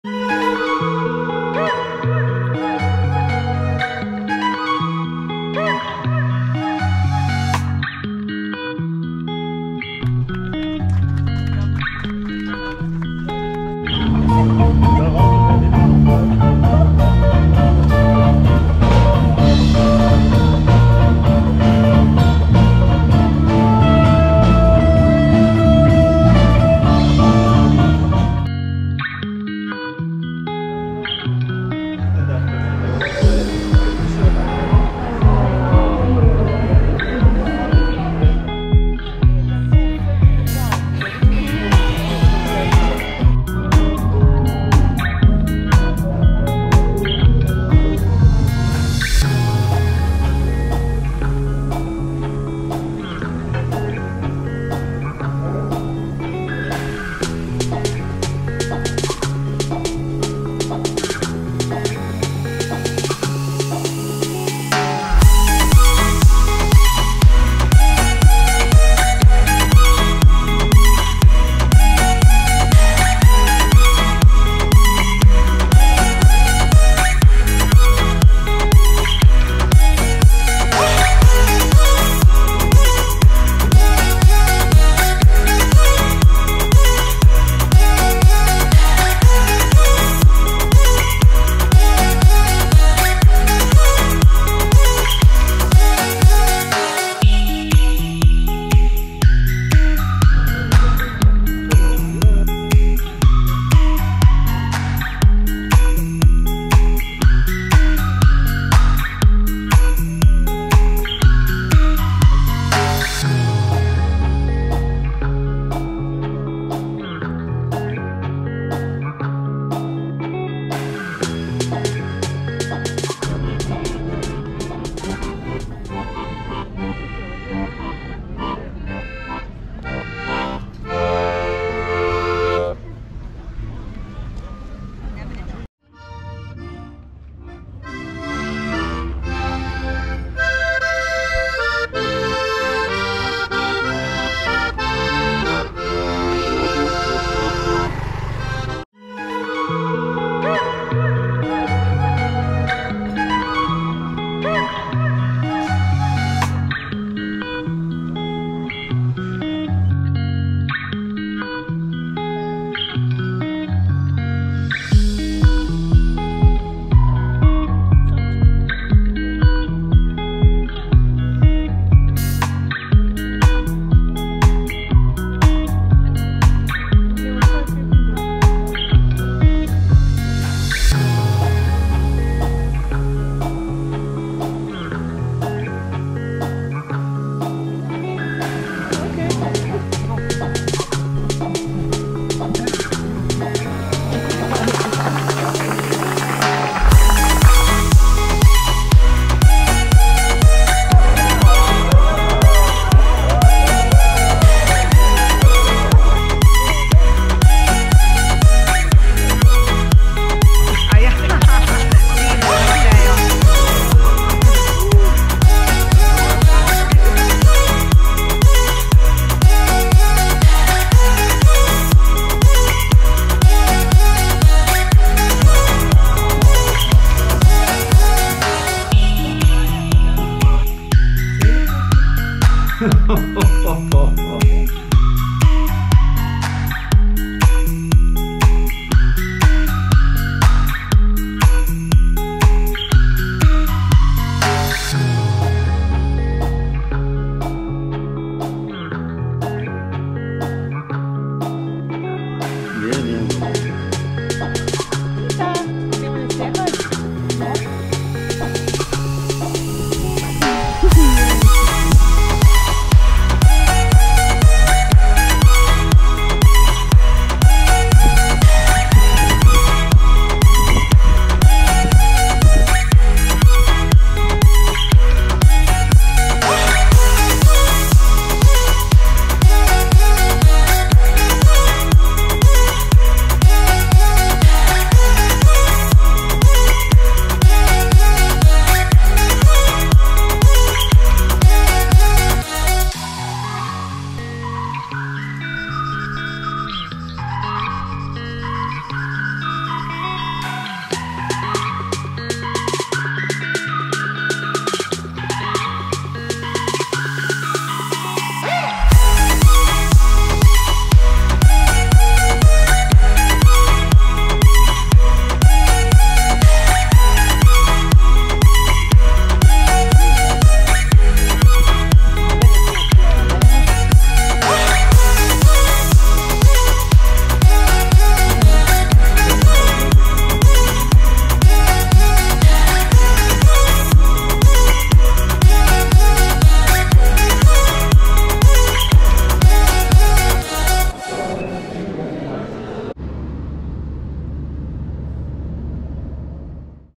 Weekend Trip to Slovenia and Croatia I Pula I Izola I Osp I Predmeja (4k)
My friends invited me to their wedding so I went. Booked the cheapest flight I could find with easyJet and had my mum waiting for me in Pula which is less then 2 hours away from my home. What matters is that I had some awesome food.